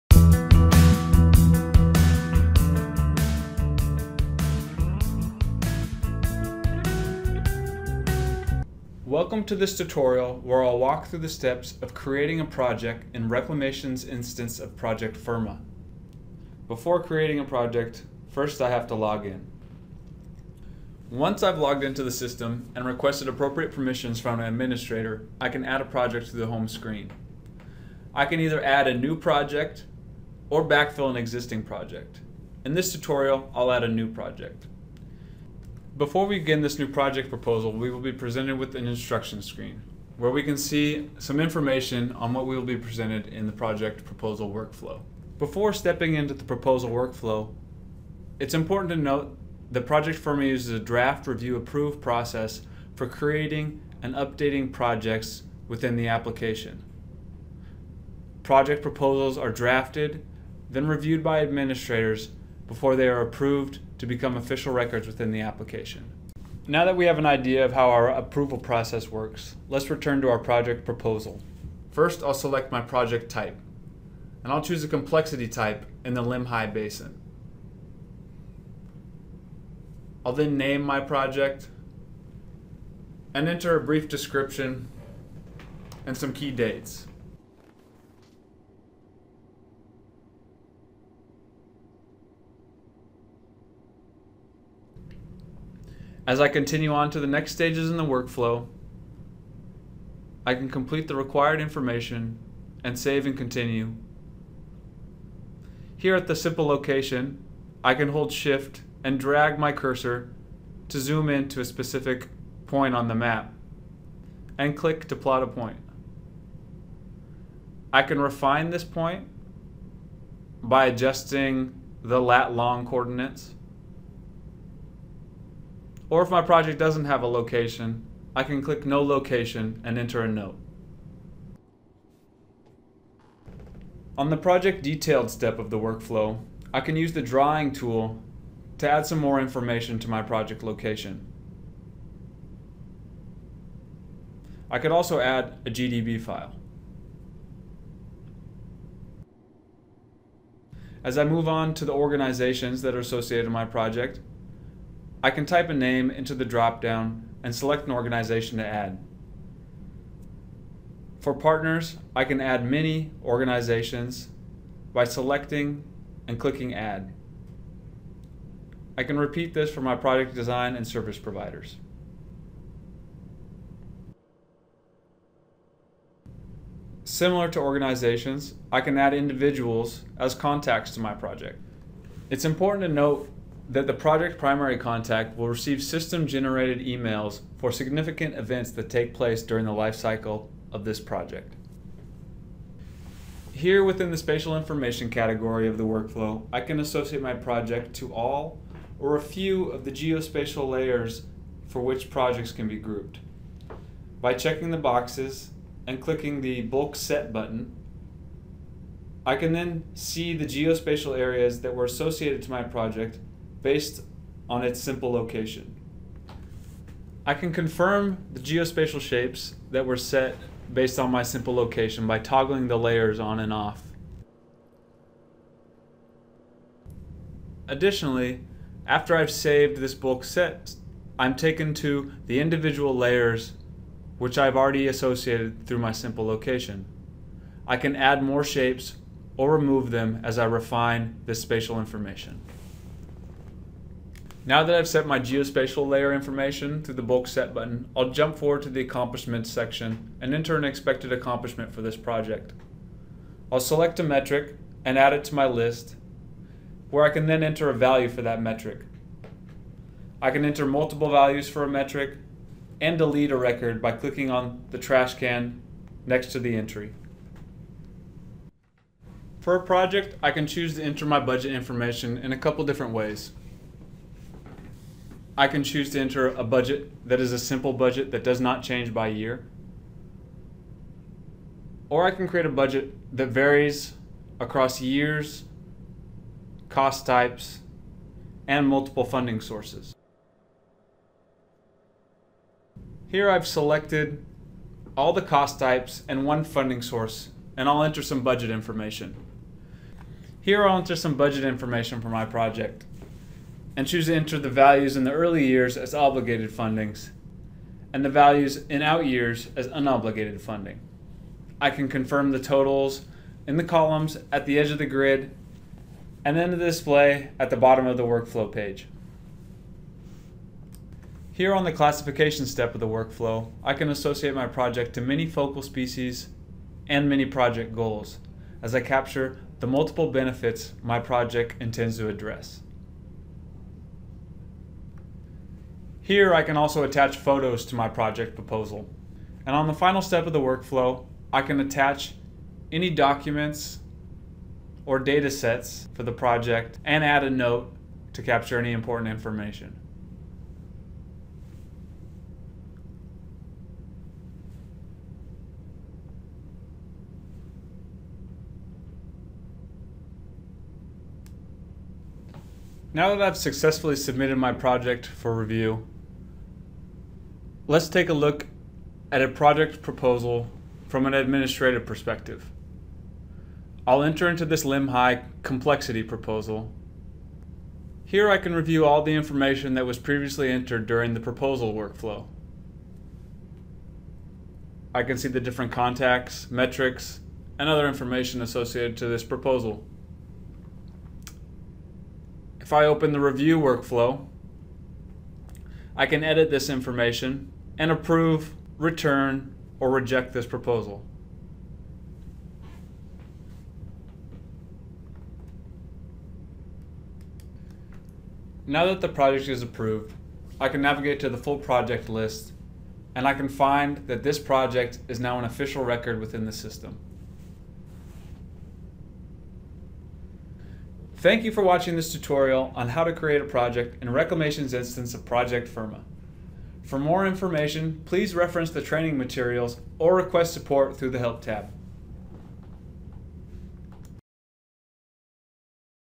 USBR ESA Database - Adding a Project Tutorial
This is the tutorial video on adding a project in US Bureau of Reclamation's instance of ProjectFirma.